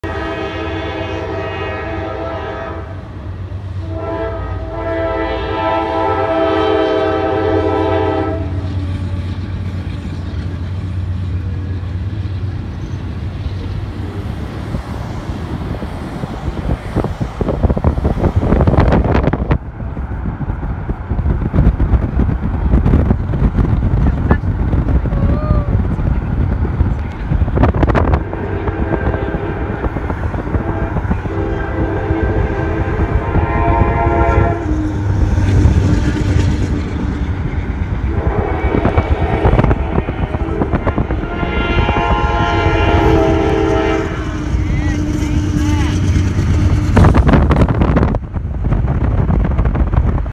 Awesome Doppler effects from CSX Q326 while doing a mini chase beside the train! (6/6/21)
Amazing Doppler effects from the K5LA horn of CSX 352 leading CSX Q326 (now M326) while heading to Tippy Cow Ice Cream on June 6, 2021 at 2:16 PM E.T. Never expected to accidentally chase a train on that day. This was also in Grandville, MI.

Main Doppler effects are at the point of 0:29 in the video. Some blasts from the horn are in the very beginning.